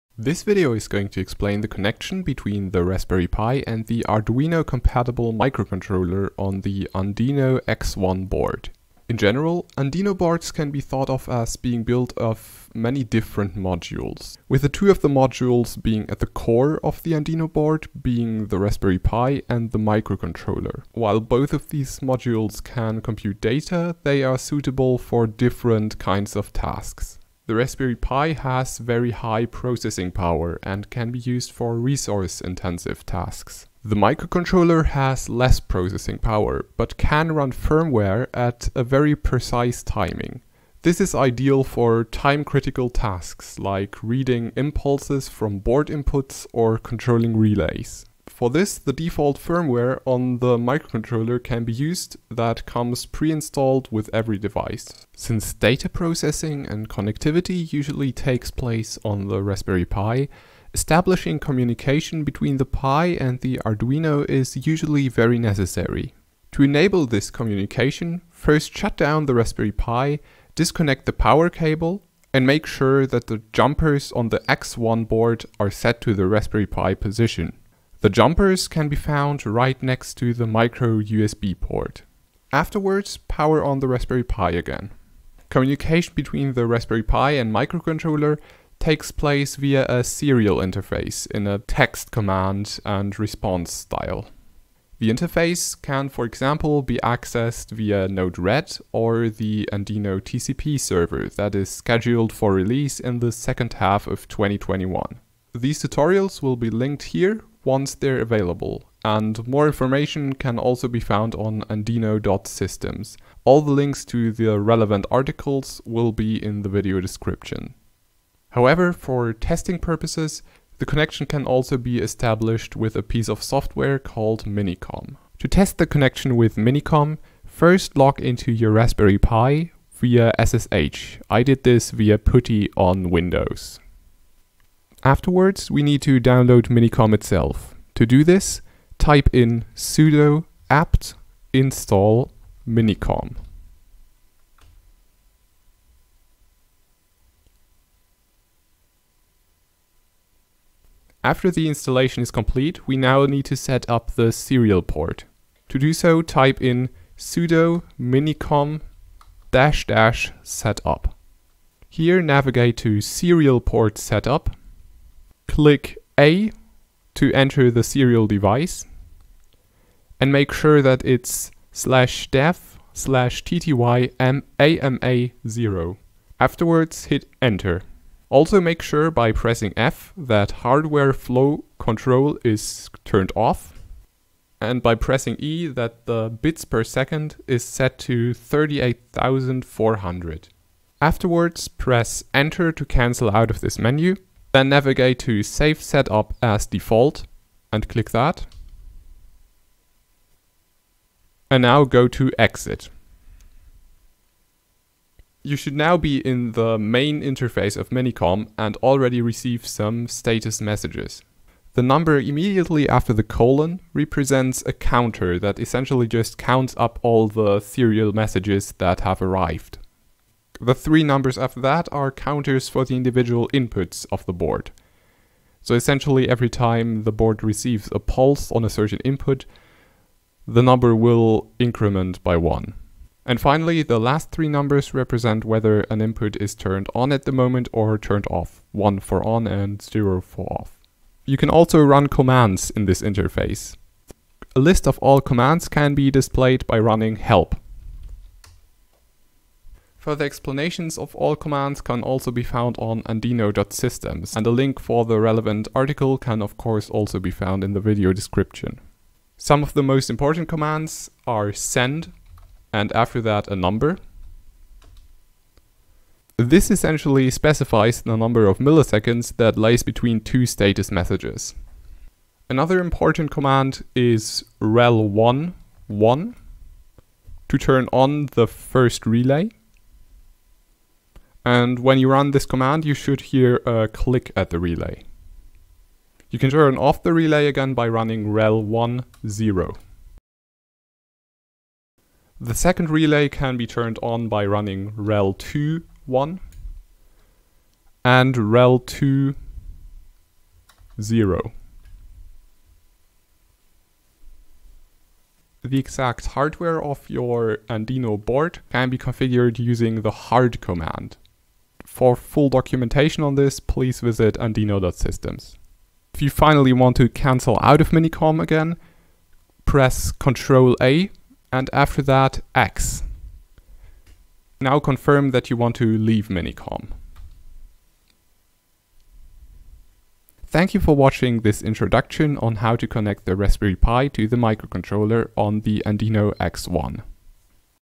How To: Connect Raspberry Pi with Arduino Microcontroller - Andino X1
This video explains the connection between the Raspberry Pi and the Arduino compatible microcontroller on the Andino X1 industrial Raspberry Pi extension board.

0:00 Connection between Raspberry Pi & Microcontroller
0:55 Establishing the connection
1:57 Testing: Minicom setup
3:23 Testing: Minicom commands